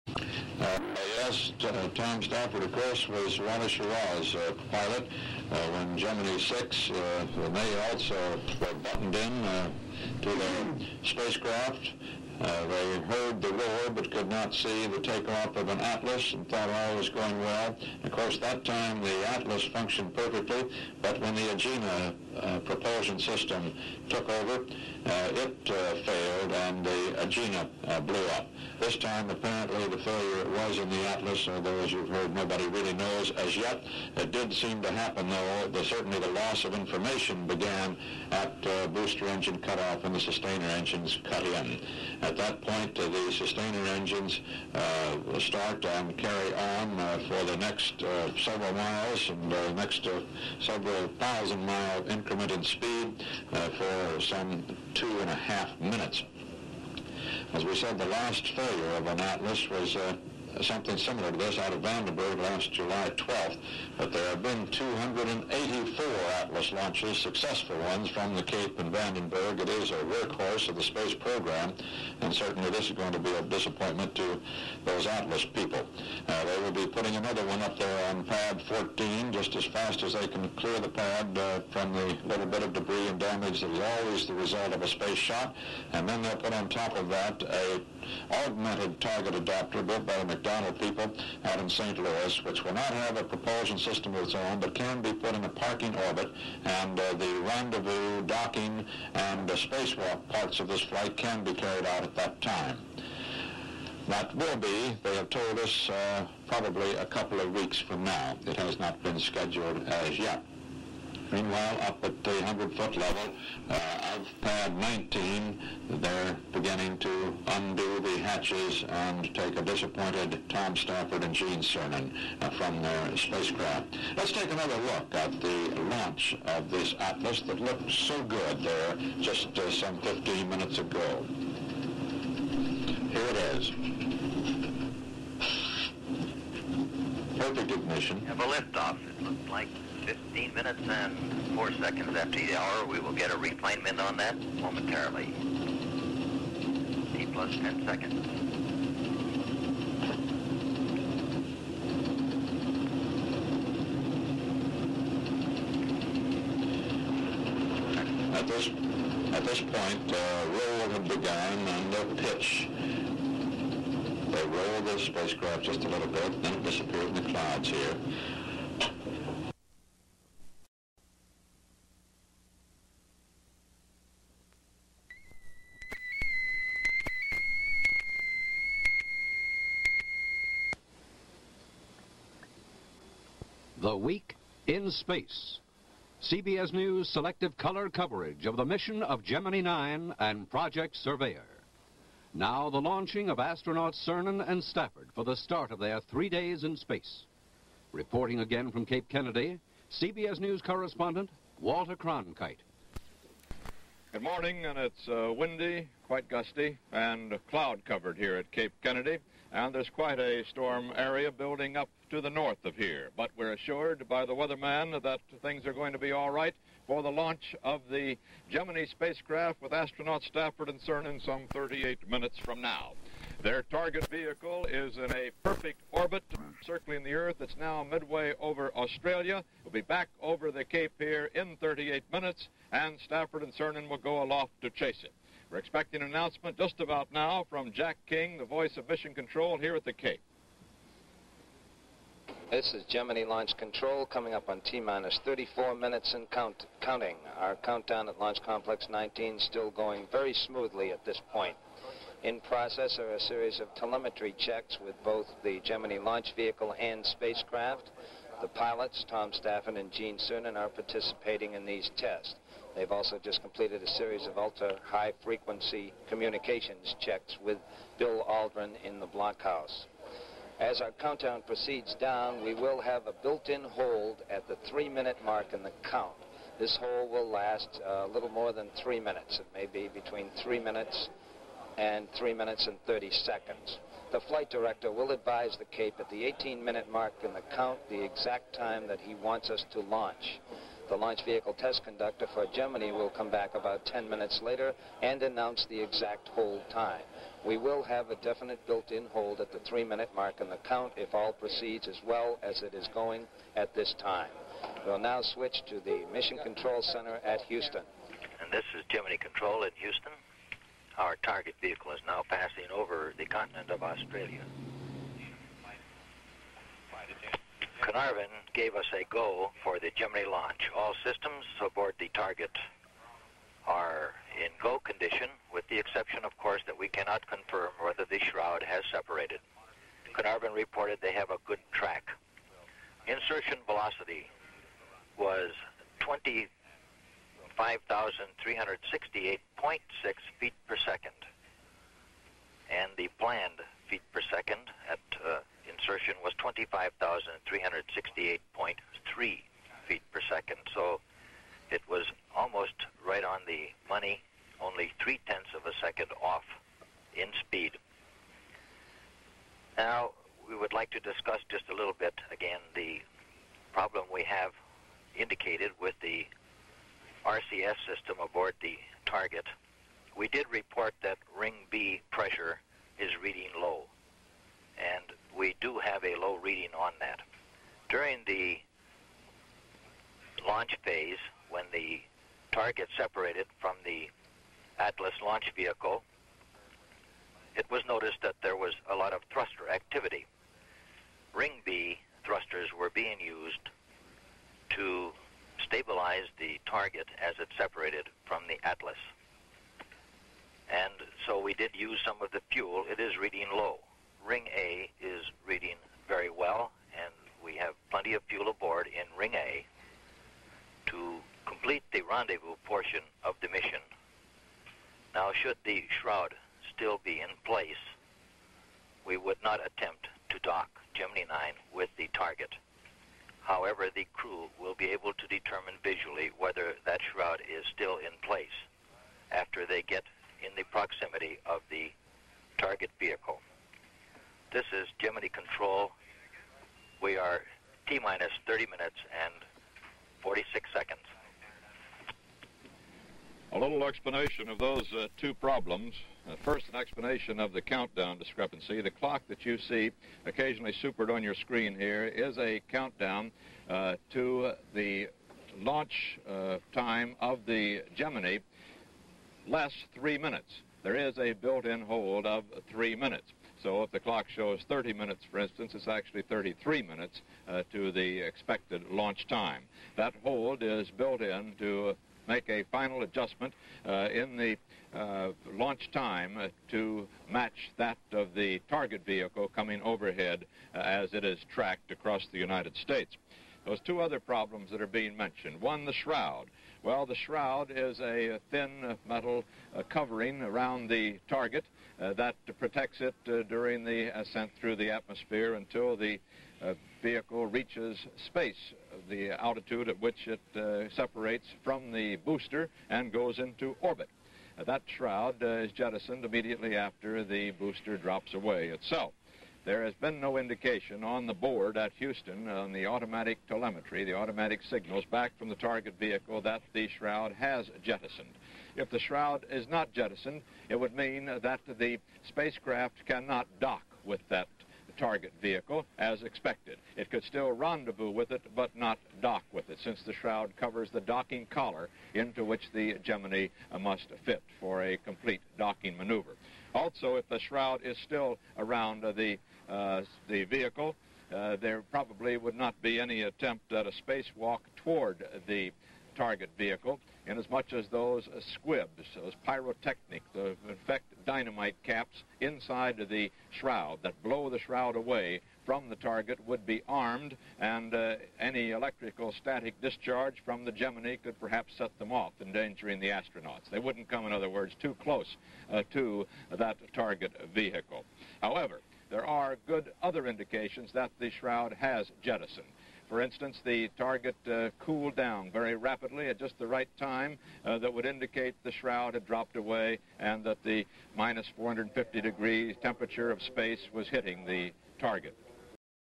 Gemni 9 Part 3
Coverage of Gemini 9 continues. The original launch is scrubbed - May 17th 1966 following the failure of the Agena to reach orbit. The second part of this segment contains the start of the 2nd launch attempt on June 1st 1966. The ATDA is in orbit and although telemetry indicates that the launch shroud is still in place, the countdown for Gemini 9 continues. There is a 40 second launch window available.